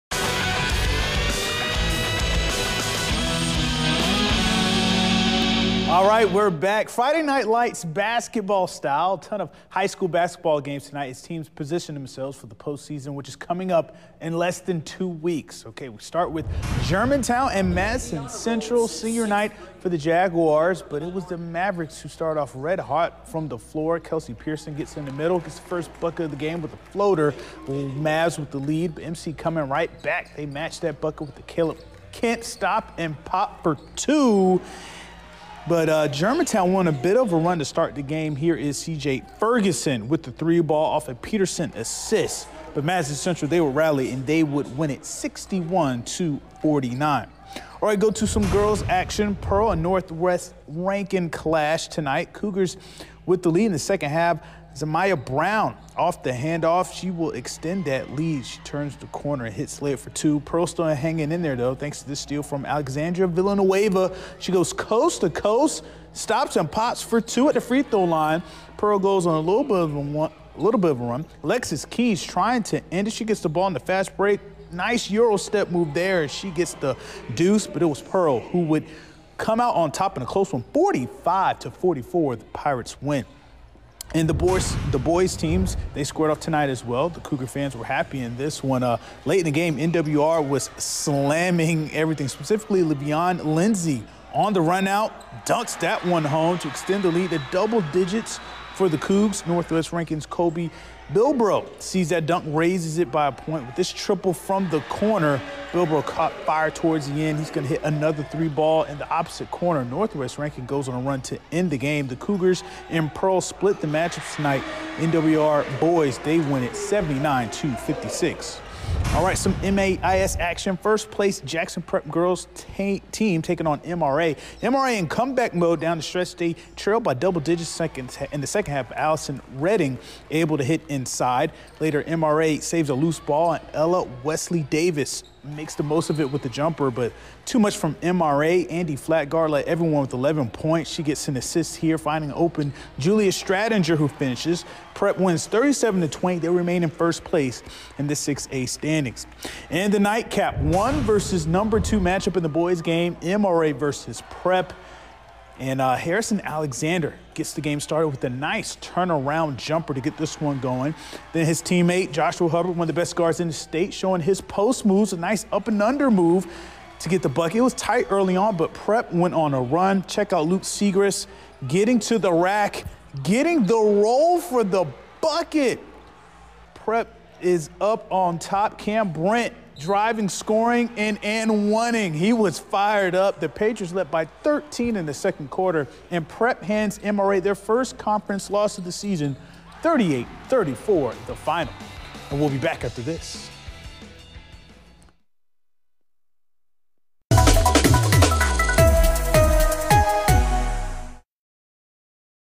Friday Night Hoops Recap
Lots of basketball action in the Metro Area Friday night as teams try to build momentum and position themselves for region and district tournaments coming up in less than two weeks.  Pearl Girls 45 Northwest Rankin 44 F Pearl Boys 56 Northwest Rankin 79 F Germantown 49 Madison Central 61 F MRA Girls 20 Jackson Prep 37 F MRA Boys 34 Jackson Prep 38 F